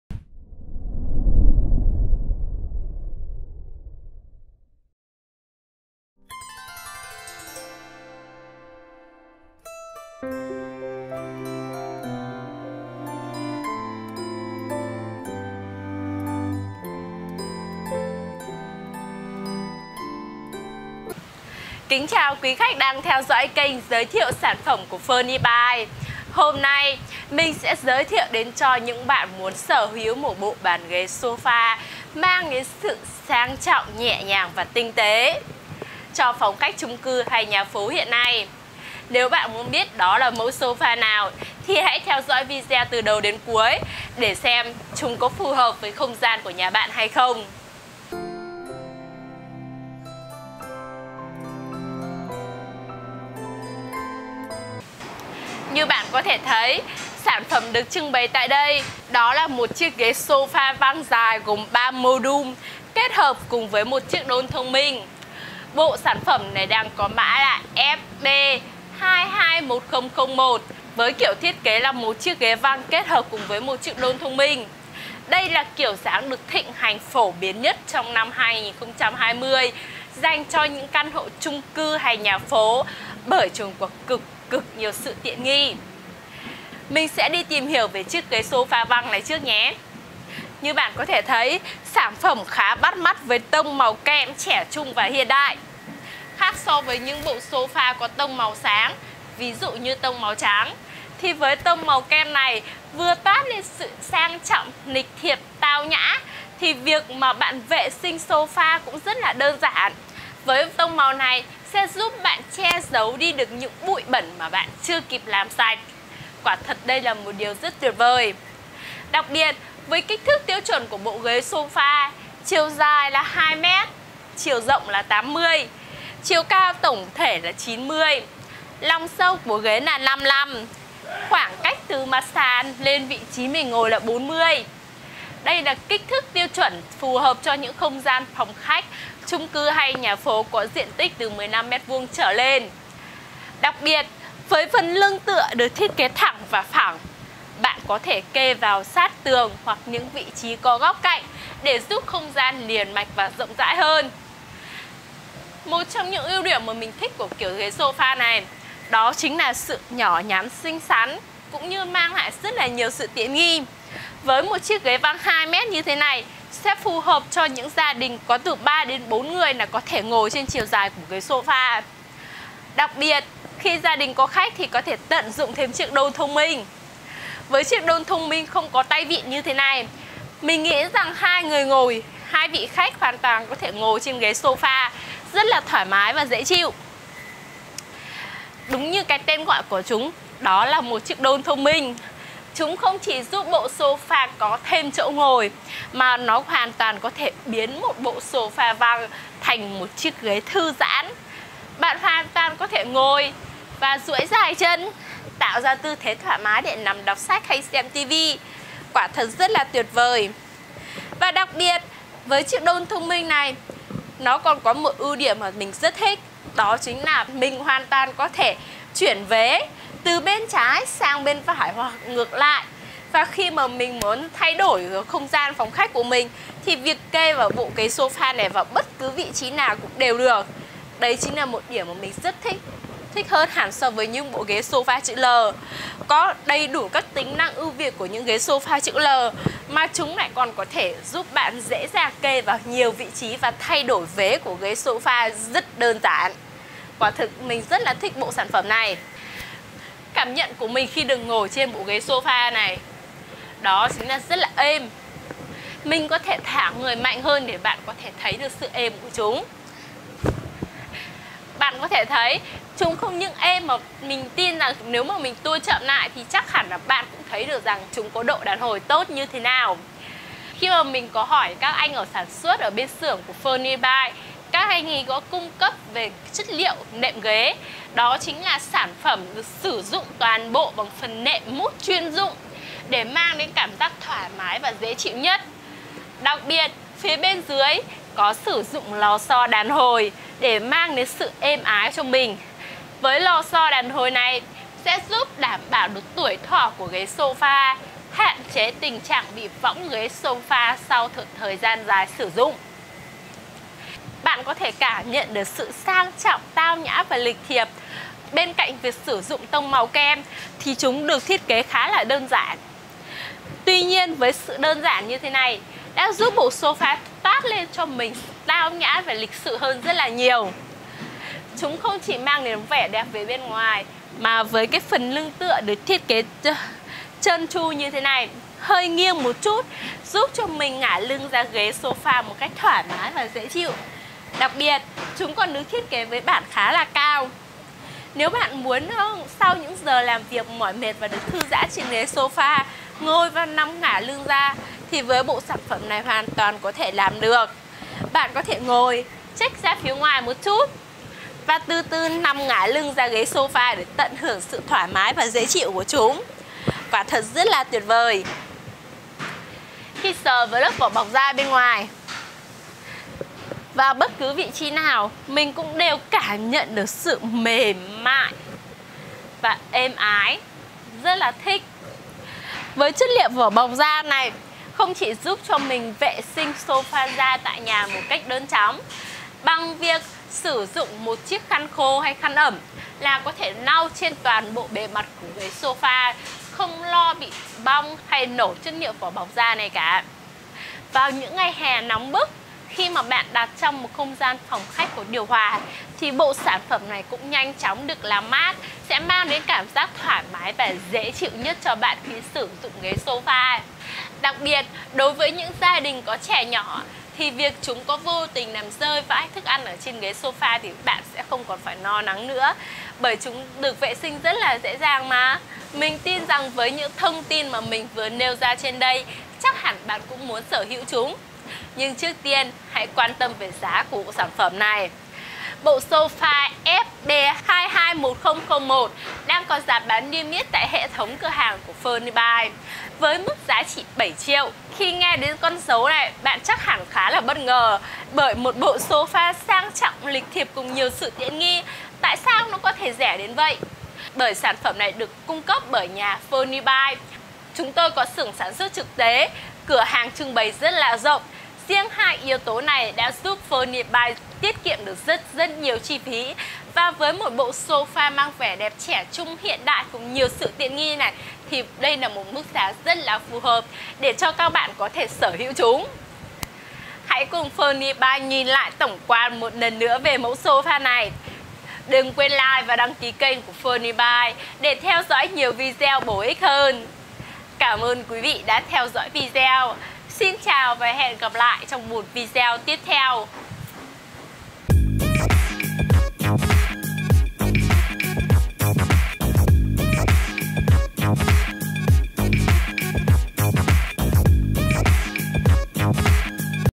Ghế sofa văng da kèm đôn lớn lắp đặt căn hộ chung cư fb 221001- Nội thất giá rẻ tại kho
Ghế sofa văng da kèm đôn lớn lắp đặt căn hộ chung cư fb 221001. Mẫu ghế được làm từ khung gỗ tự nhiên, lớp vải áo da nhập khẩu cao cấp. Nệm mút dày dặn, êm ái. Để biết nhiều hơn các thông tin về mẫu sofa này như: Kích thước sofa, chất liệu sofa, giá bán sofa, địa chỉ bán sofa thì hãy để ra vài phút để theo dõi video này nhé!